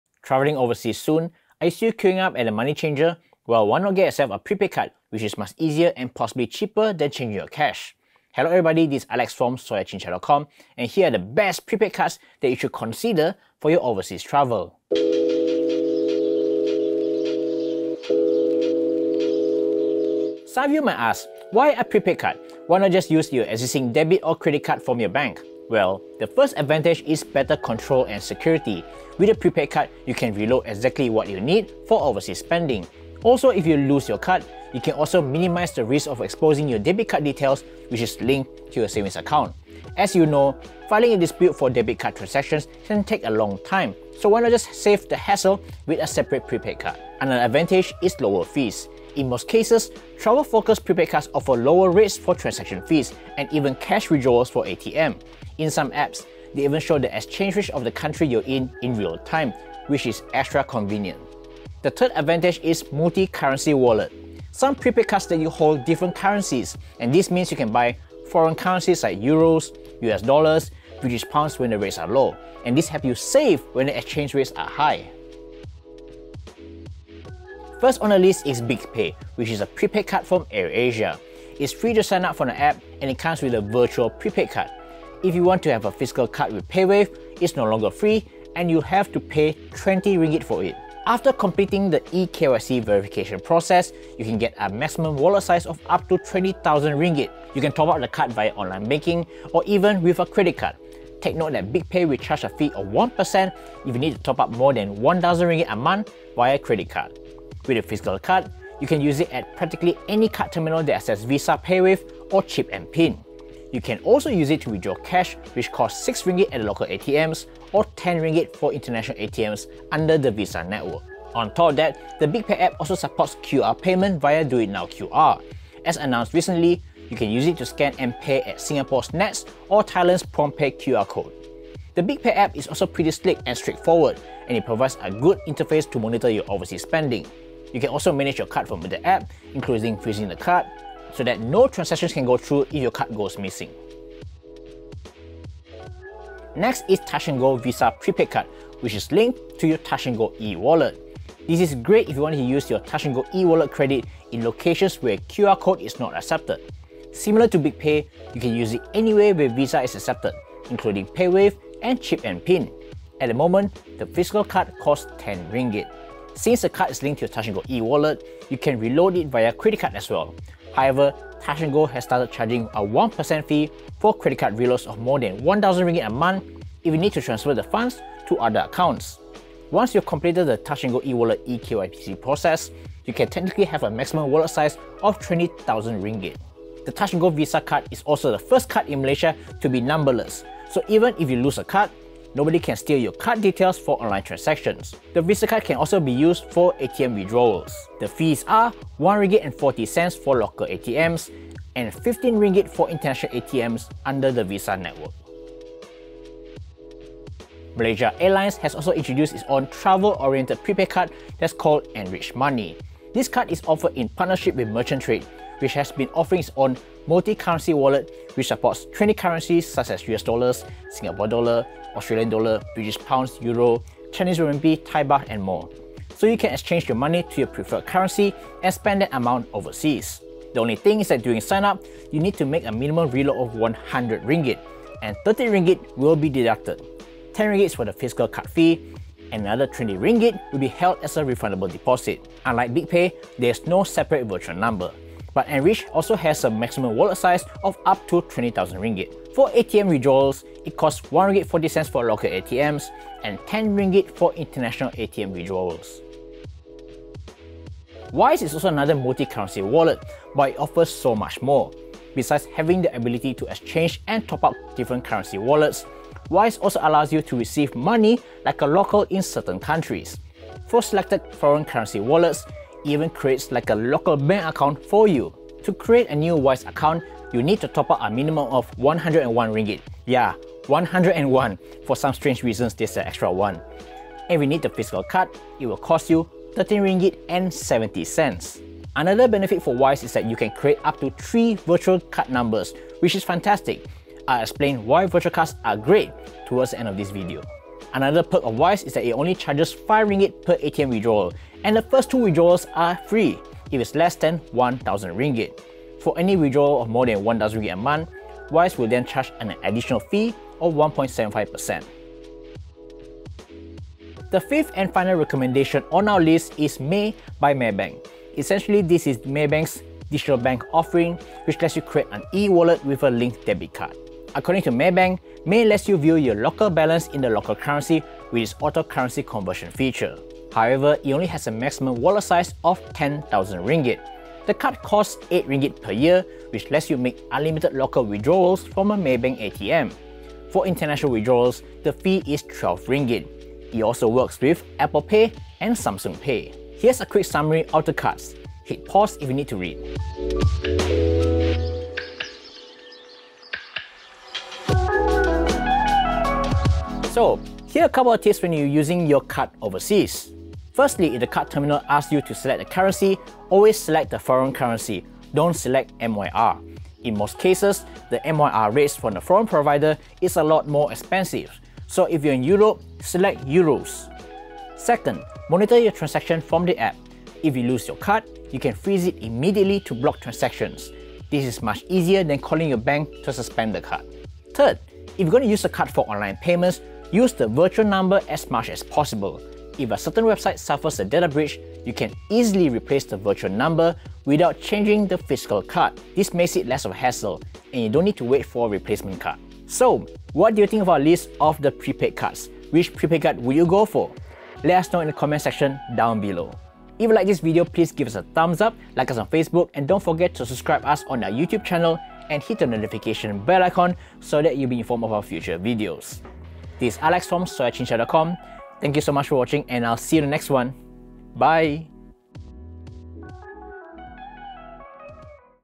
Best Visa Prepaid Cards for Overseas Travel | BigPay, TNG, EnrichMoney, Wise and MAE
Travelling overseas soon? Using Prepaid Cards for your travel could be a better option than buying foreign currency or using credit and debit cards. Besides better financial control, several travel-focused prepaid cards offer extra features such as virtual card numbers, competitive foreign exchange rates and cheaper ATM withdrawal fees. We compare BigPay, Wise, EnrichMoney, Touch 'n Go Visa and MAE by Maybank.

CHAPTERS

0:00 - Intro
0:18 - Why use a prepaid card for overseas spending?
1:15 - BigPay
2:25 - Touch 'n Go eWallet Visa
3:26 - EnrichMoney by Malaysia Airlines
4:16 - Wise
5:12 - MAE by Maybank
5:46 - Summary of Prepaid Cards in Malaysia
5:55 - Travel Tips for making payment overseas
6:39 - End